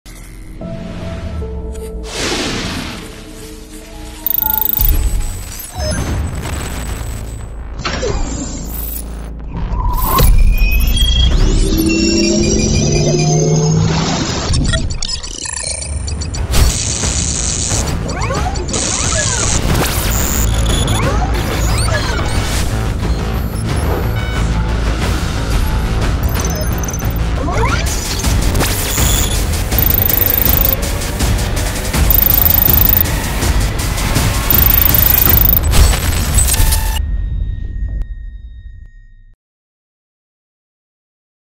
Zoomlion Bilim ve Teknoloji Fuarı için geri sayım başladı!
Zoomlion'ın en yeni ürün ve teknolojilerini , insansız akıllı üretim sahalarını tanıtacağı  Zoomlion Bilim ve Teknoloji Fuarı için geri sayım başladı! Son 7 gün !!!